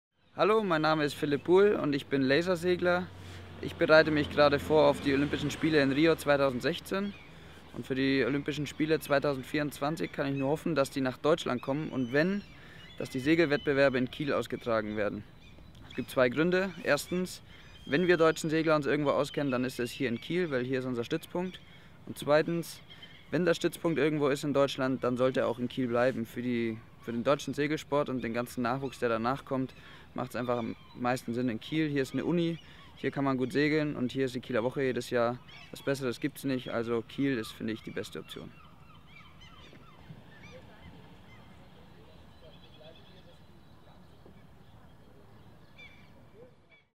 kielfuerolympia - Lasersegler Philipp Buhl sagt, warum Olympia nach Kiel muss
Lasersegler Philipp Buhl sagt, warum Olympia nach Kiel muss.